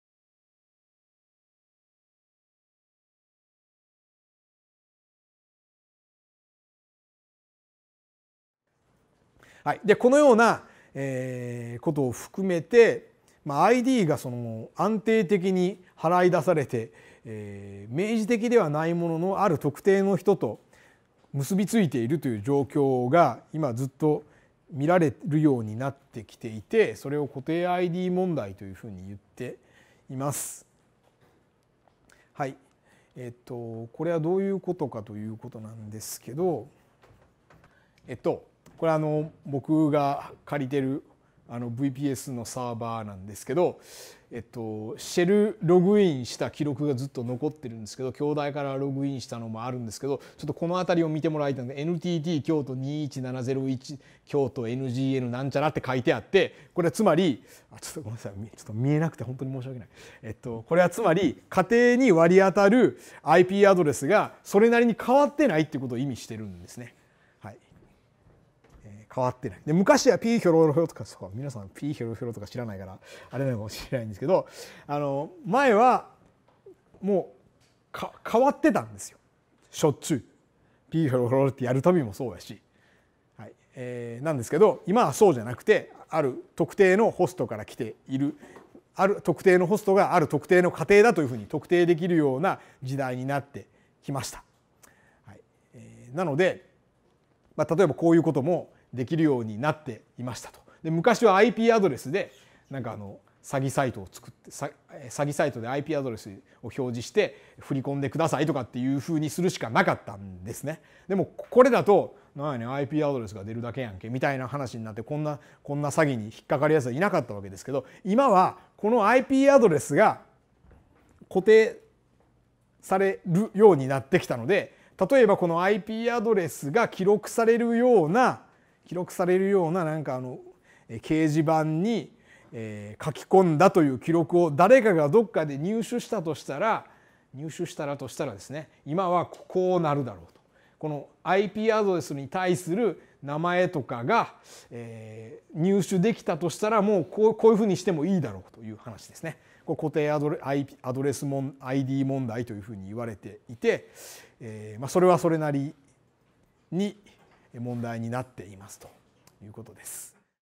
京都大学大学院情報学研究科「情報学展望1 情報セキュリティ」上田 浩 准教授 2015年6月26日-08
京都大学OCWから配信されています。
2015年6月26日 「トレーサビリティとプライバシー」 上田 浩 准教授

他のチャプターはこちらからご覧いただけます。